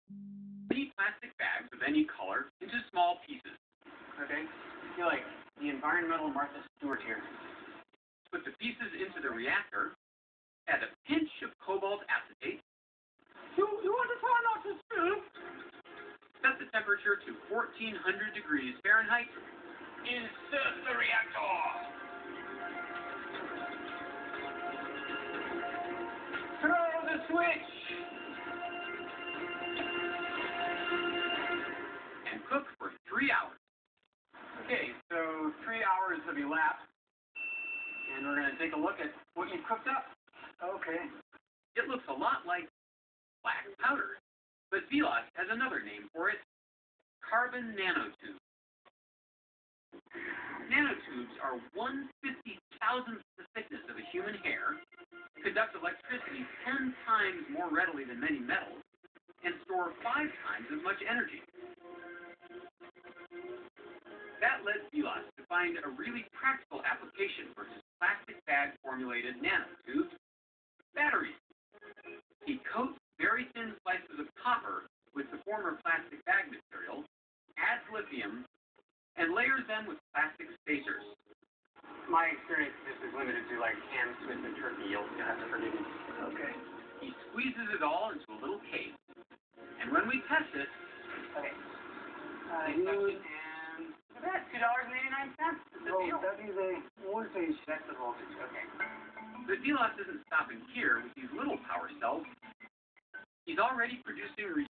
Plastic Bag Cobalt Acetate Carbon Nanotube Battery with Lithium
nanotubes conduct electricity ten times better than metal and store five times more energy